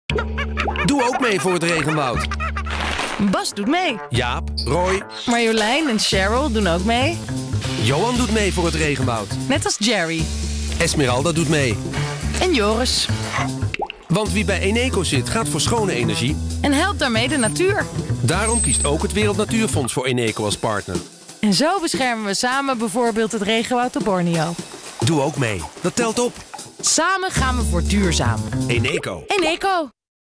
Eneco douchen
Doe ook mee voor het regenwoud. Want wie bij Eneco zit gaat voor schone energie. En helpt daarmee de natuur. Daarom kiest ook het Wereld Natuur Fonds Eneco als partner. En zo beschermen we samen bij het regenwoud op Borneo. Doe ook mee, dat telt op. Samen gaan we voor duurzaam.